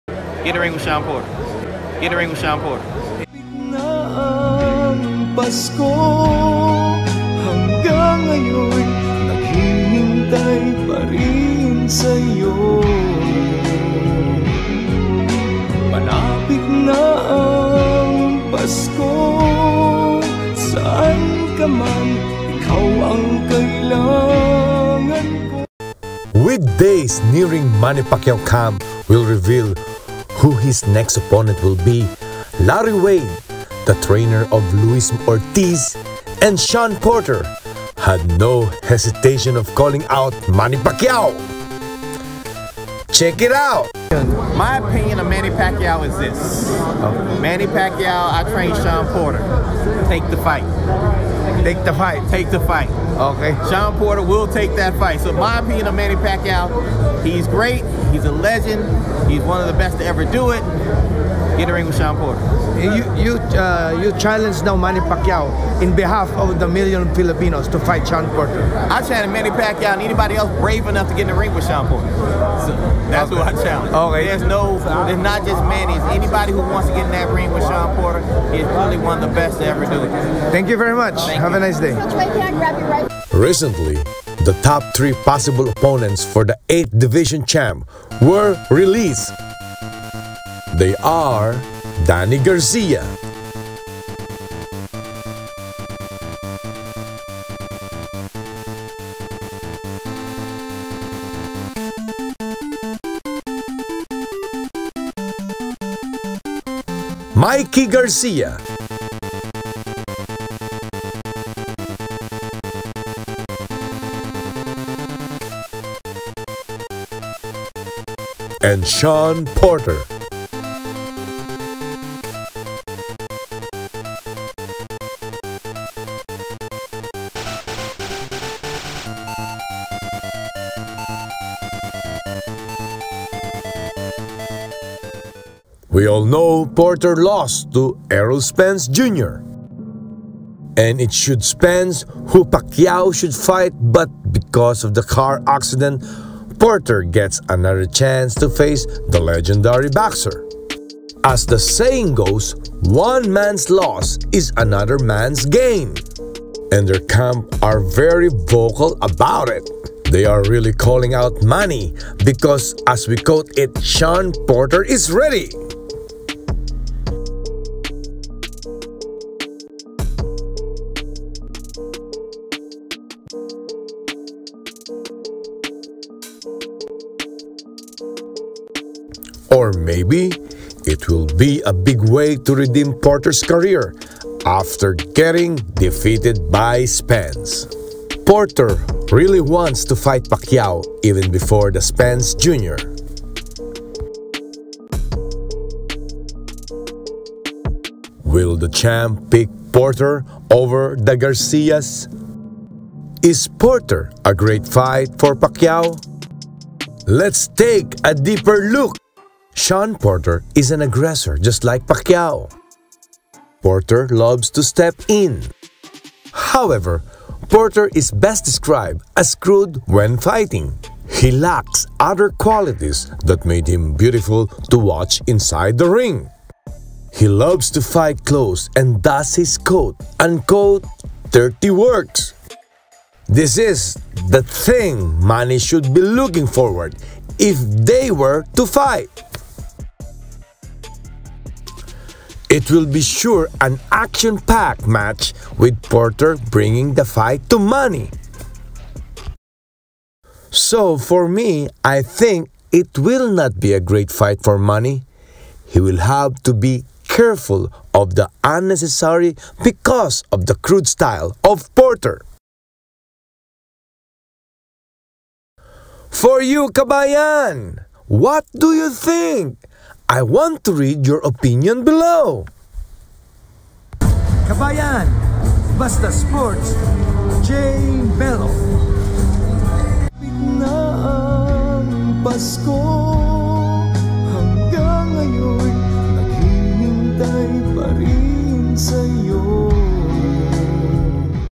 ***Manny Pacquiao "GET in the RING with PORTER!"
Head trainer of Shawn Porter dares Pacquiao to pick Porter! Check it out!